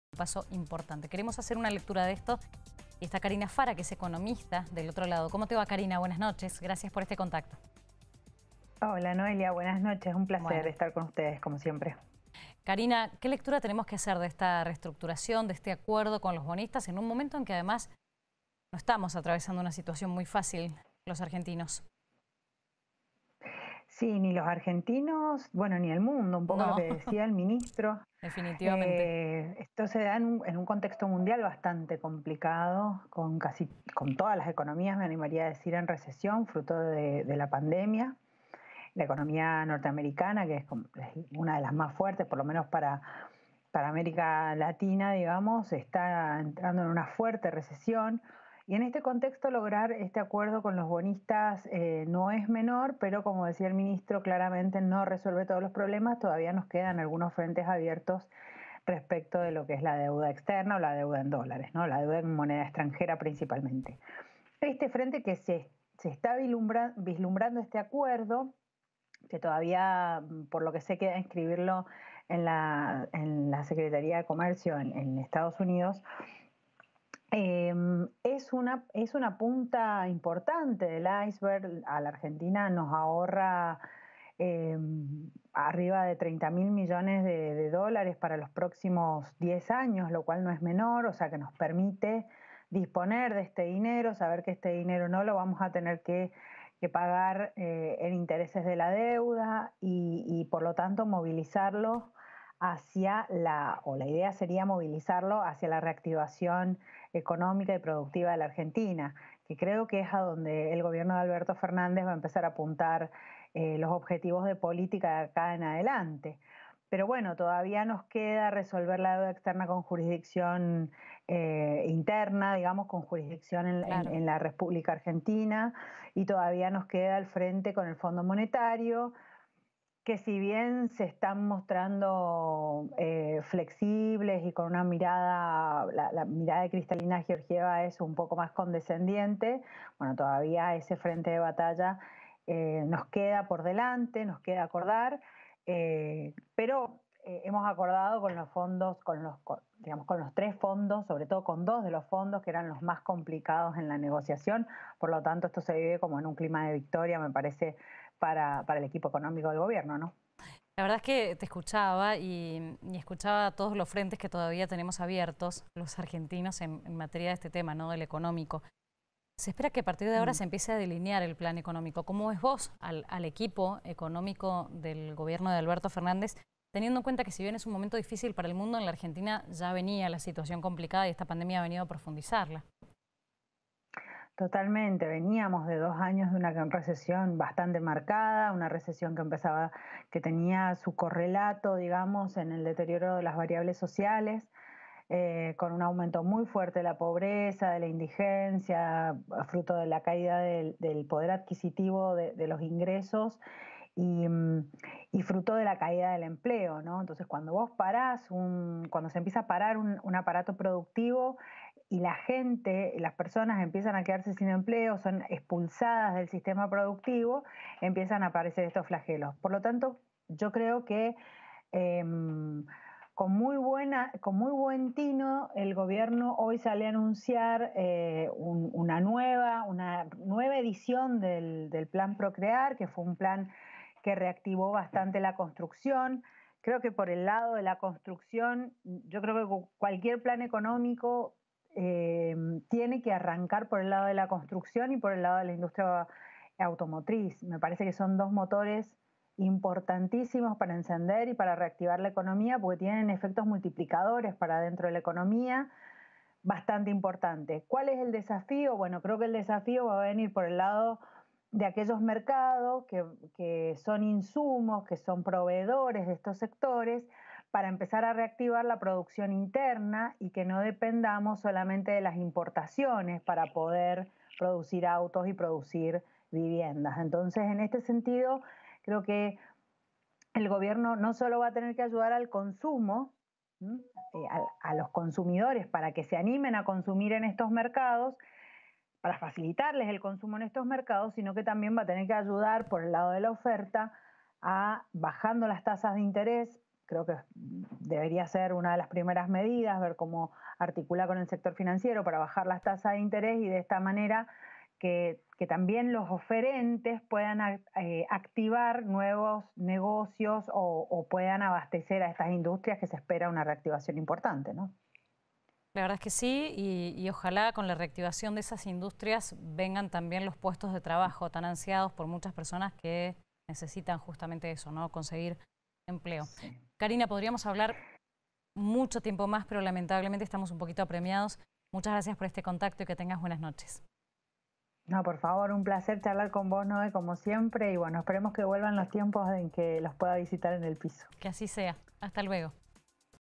RESTRUCTURACIÓN DE LA DEUDA
El Gobierno confirmó el acuerdo con los Bonistas de Wall Street